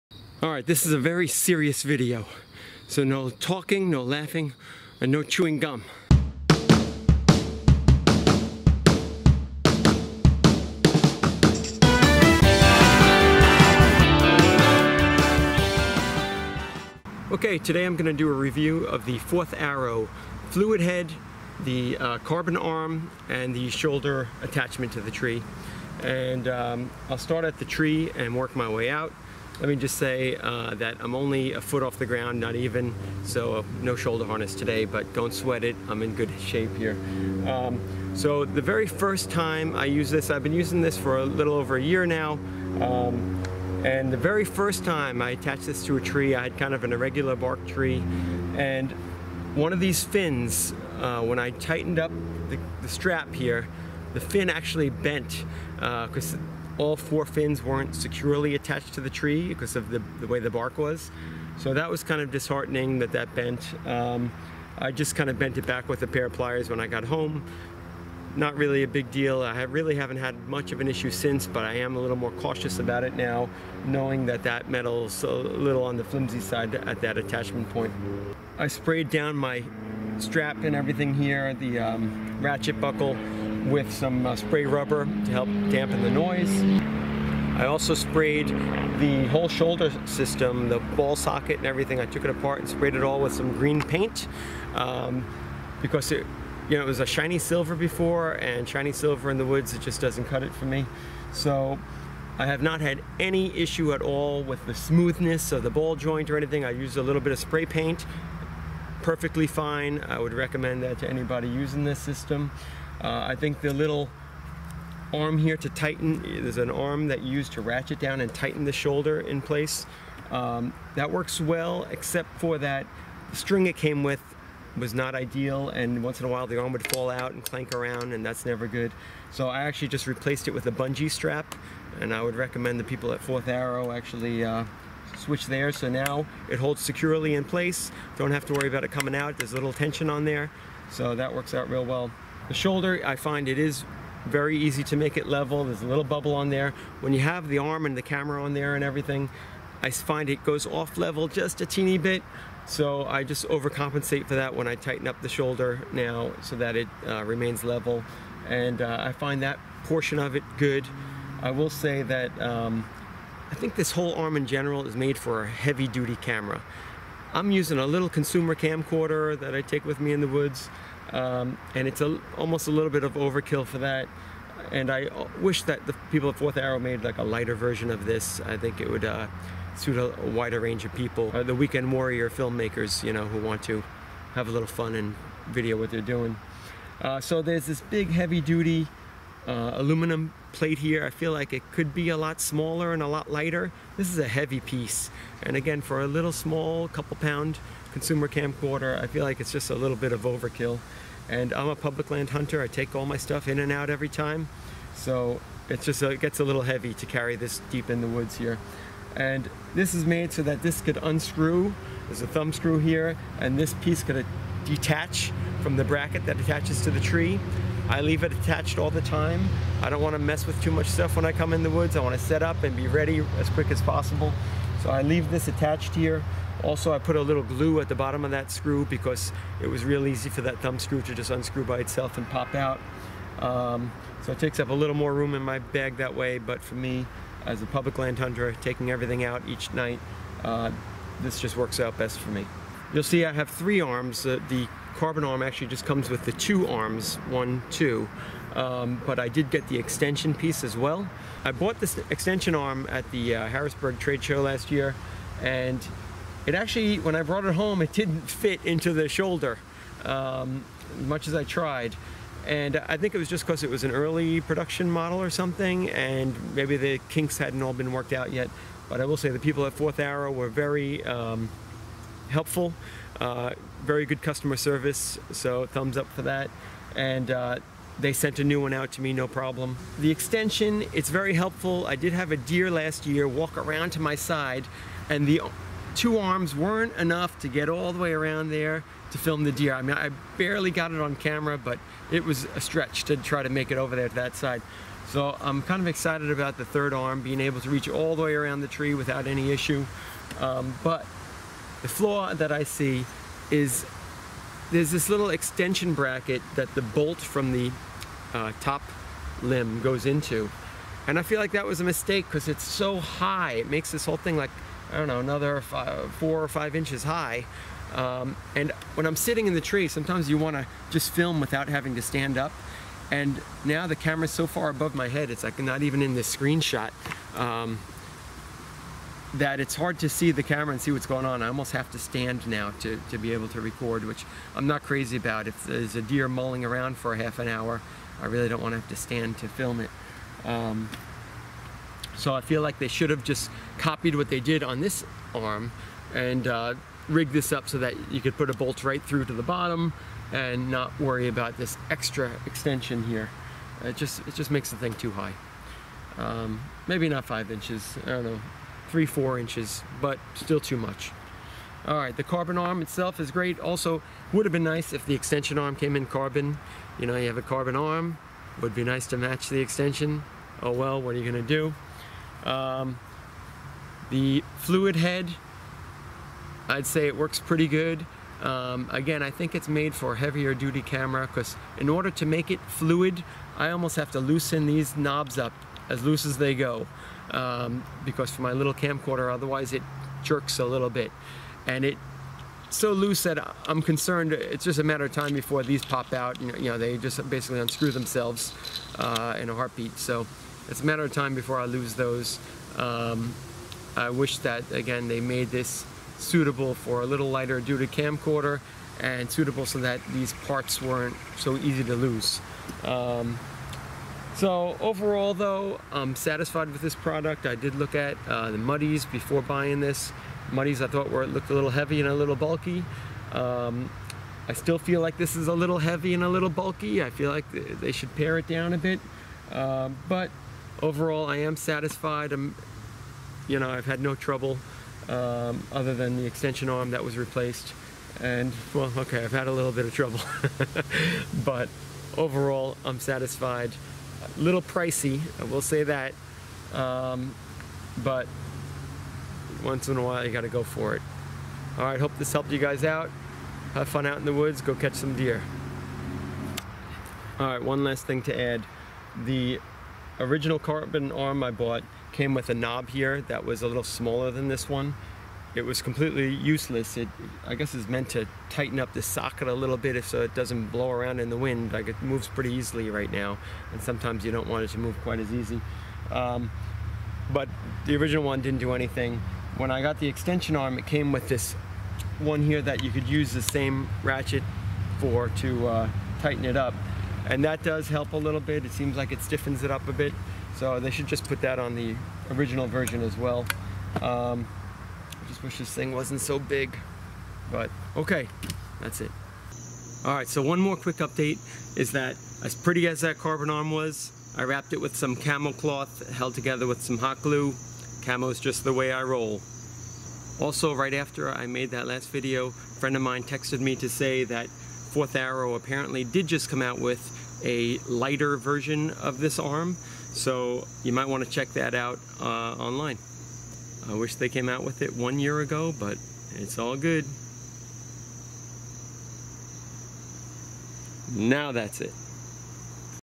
Fourth Arrow Carbon Arm, Shoulder, Extension, and Fluid Head Review - Vital Point
This is a review of the Fourth Arrow system after using it for self-filming bowhunting all last season.   Hope it helps you to make an informed purchase decision.